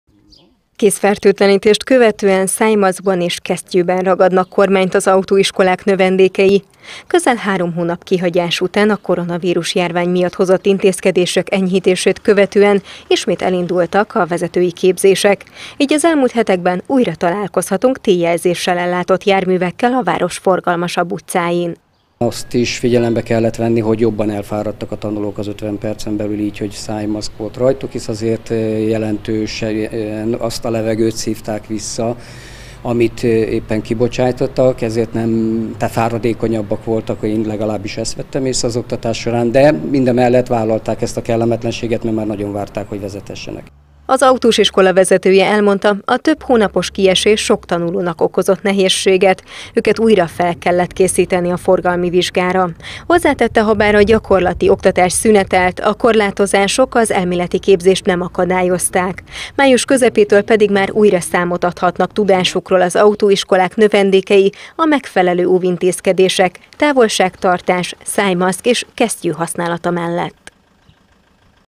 Újra találkozhatunk az utakon tanulóvezetőkkel - 2020.06.16.
Közel három hónap szünet után ez elmúlt hetekben újra elkezdődött a gyakorlati oktatás az autósiskoláknál. A koronavírus-járványra való tekintettel szájmaszkban és kesztyűben kell bizonyítaniuk a tanulóknak.